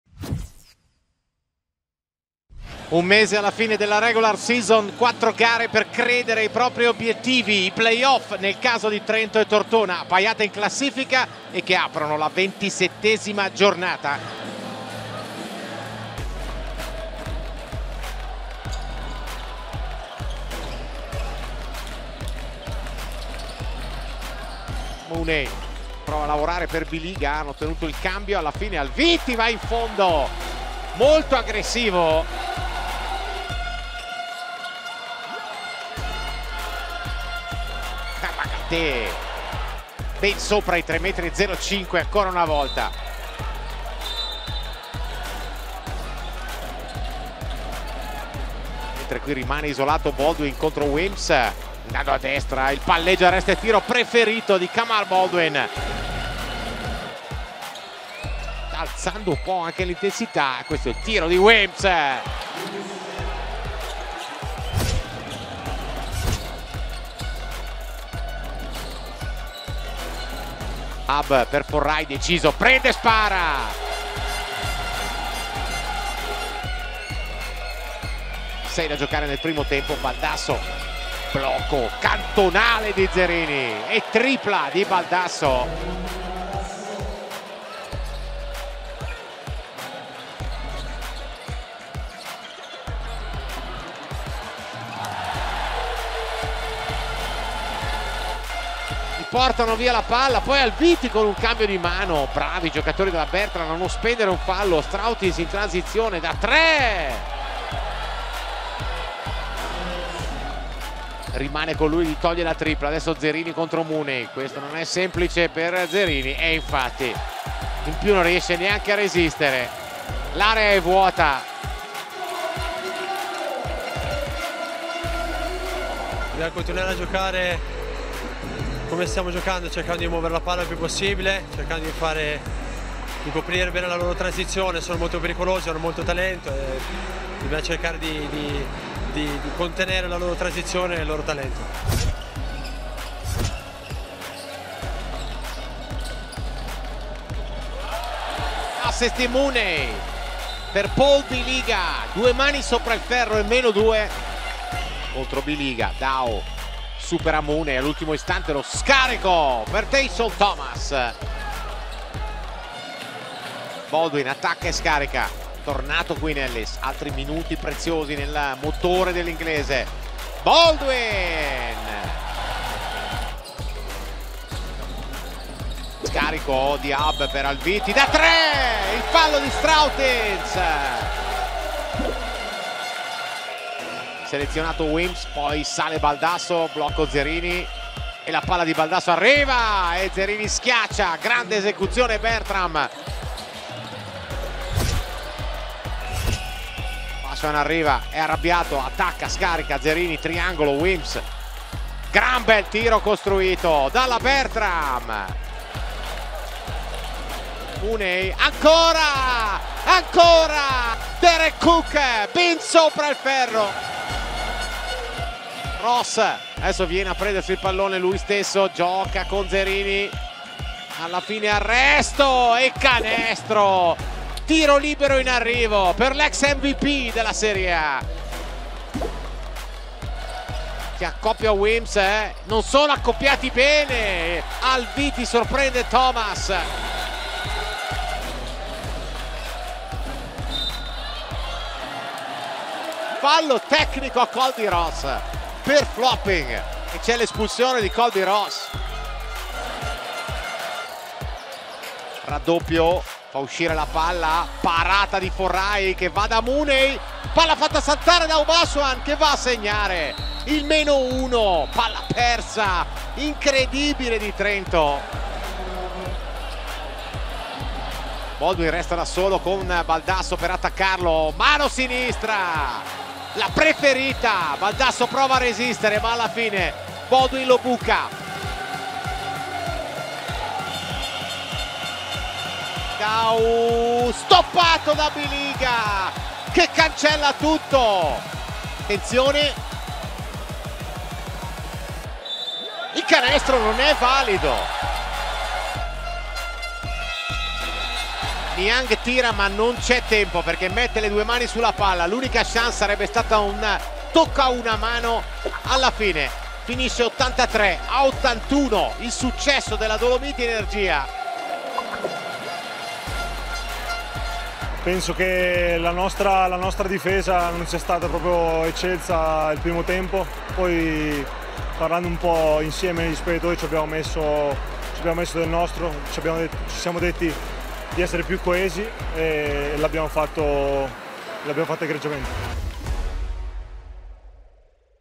Dolomiti Energia Trentino - Bertram Derthona Tortona | Serie A UnipolSai 2023/24
Gli highlights della sfida tra Trento e Tortona, partita valida per il 27° turno di campionato della Serie A UnipolSai 2023/24.